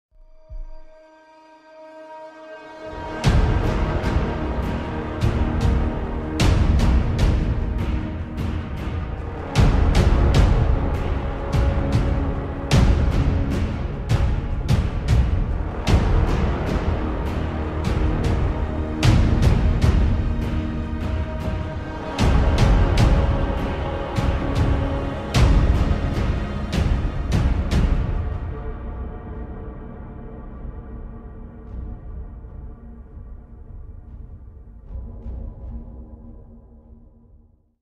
Pink/Lilac Dragon 3D Print Time-lapse - Elegoo Neptune 3 Pro
Testing Klipper on my Elegoo Neptune 3 Pro using a Rainbow Silk PLA. Faster print times and like my Sonic Pad (so Klipper) a ton of control. Octoprint while leveraging near stock Marlin is great, but there is a noticeable bump with Klipper and after doing some input shaping - just ordered a USB device that should further improve the process like the Sonic Pad does. Didn't really want to mess with any first layer stuff so just put in a cheat code and printed a raft.